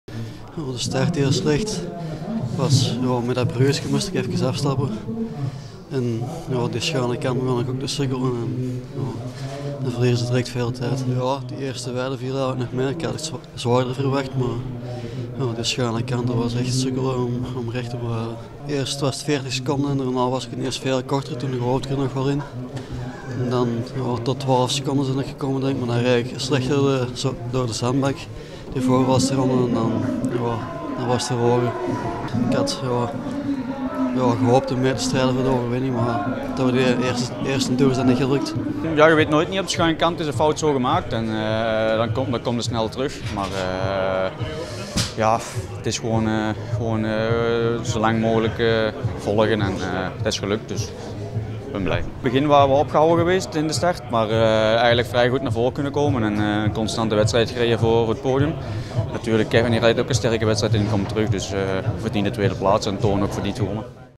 Reacties Kevin Pauwels en Corné Van Kessel na Jaarmarktcross (Soudal Classics)
Kevin Pauwels en Corné Van Kessel konden de kloof tussen Toon Aerts niet dichtrijden en eindigden zo respectievelijk tweede en derde. Bekijk hier hun reacties.